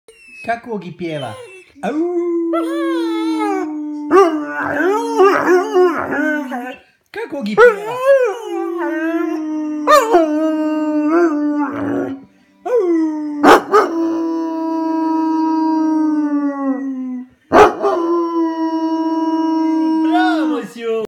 Weimaraner world champion singer
I'm Oggie, one year old weimaraner and I love to sing.